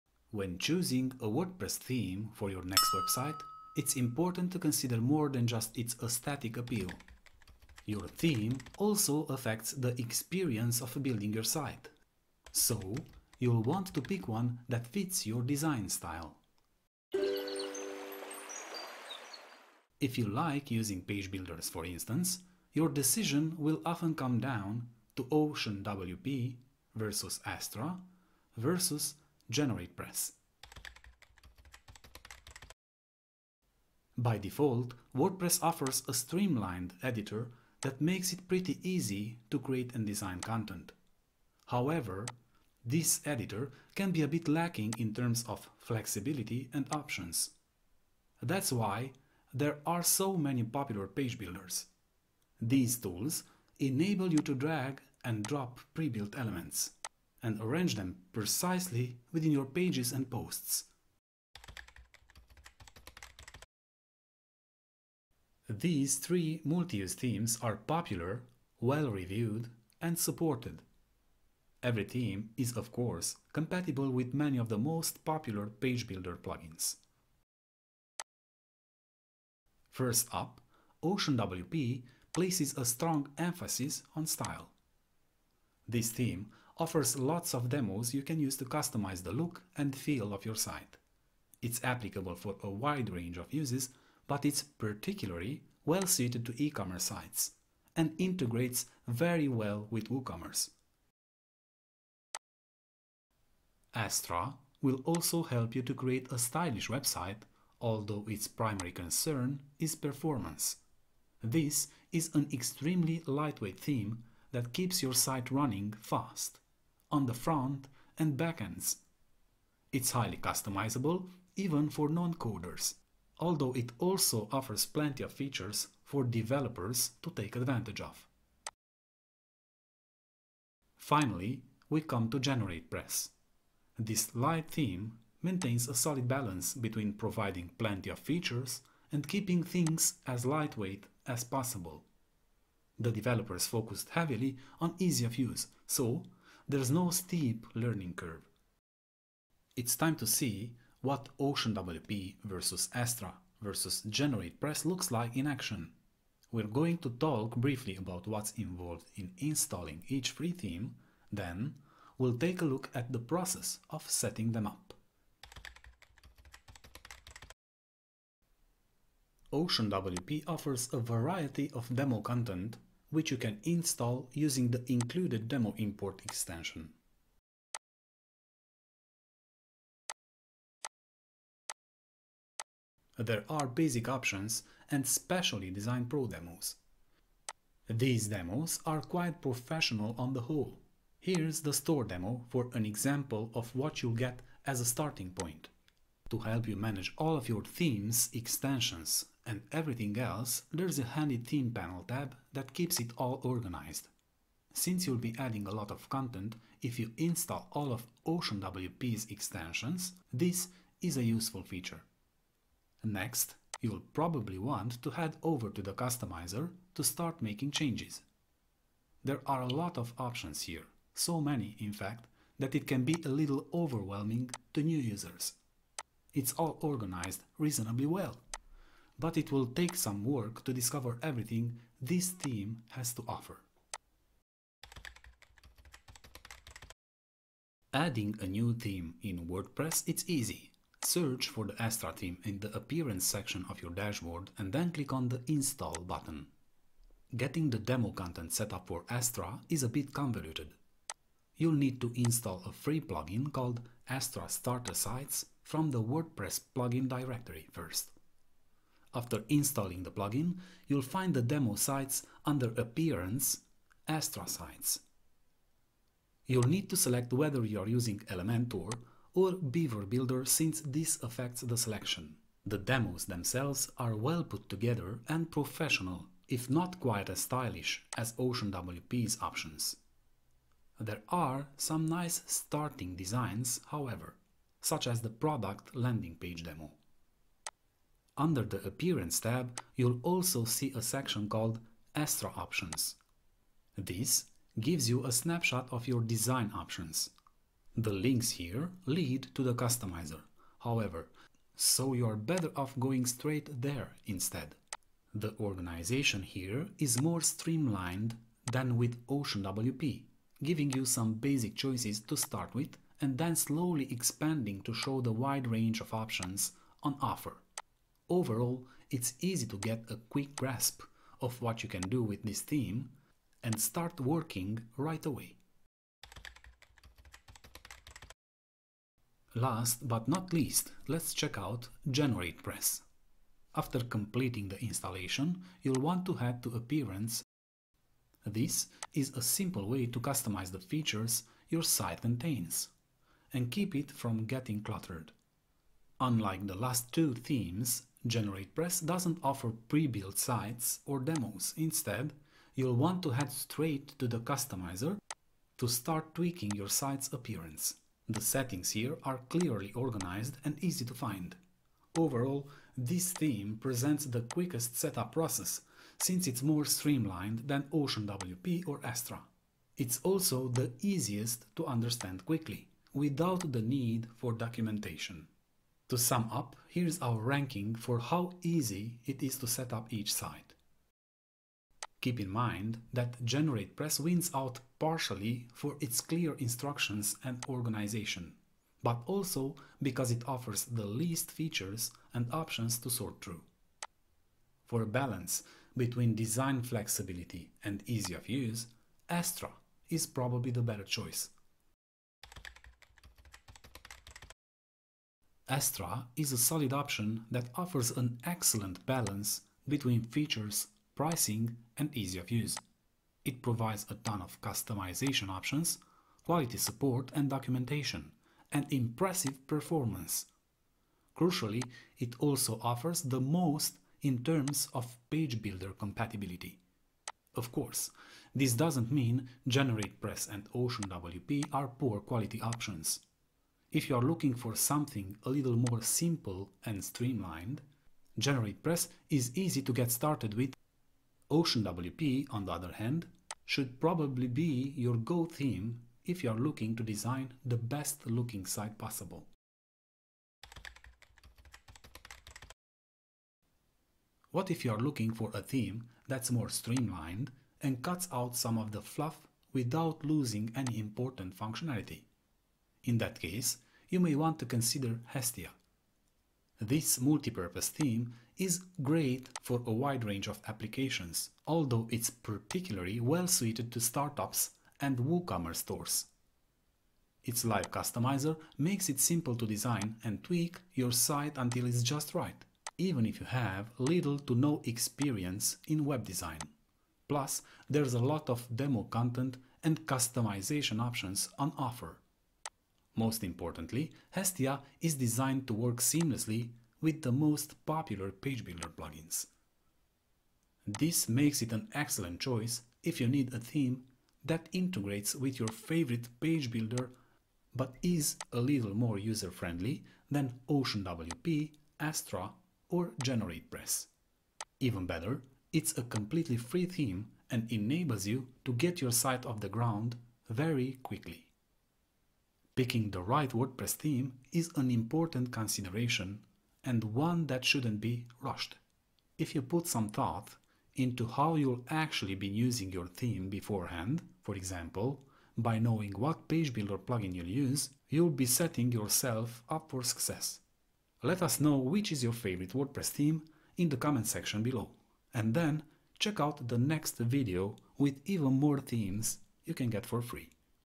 Oceanwp Vs Astra Vs Generatepress: Best Theme For Page Builders
If you're a fan of using page builders to design your websites, you'll want to choose a theme that's compatible with your favorite tools. In this comprehensive review, we'll compare three of the most popular WordPress themes to use alongside page builders.

0:32 | Why Page Builder Themes
2:46 | Install OceanWP
3:56 | Install Astra
5:28 | Install GeneratePress

Picking the right WordPress theme is an important consideration and one that shouldn’t be rushed. If you put some thought into how you’ll actually be using your theme beforehand – for example, by knowing what page builder plugin you’ll use – you’ll be setting yourself up for success.

In this review, we compared three of the most popular and feature-rich WordPress themes that are perfect for page builder enthusiasts. To recap, here are our conclusions about OceanWP vs Astra vs GeneratePress:

Astra: This was the best overall theme in our review, offering lots of features, excellent performance, and compatibility with a wide selection of popular page builders.
GeneratePress: As an alternative option, this is a lightweight, easy-to-use theme that’s well priced and very streamlined.
OceanWP: Although it’s a bit more convoluted to use, this theme offers a lot of options, and places a strong focus on helping you create beautiful designs.
Hestia: An easier-to-get-started-with alternative than the above – simply giving you a theme that works and looks great out the box – while also being compatible with the popular page builder plugins.

Have you used any of these themes? If so, what did you think of them? Who’s the winner between OceanWP vs Astra vs GeneratePress for you? Share your experiences with us in the comments section below!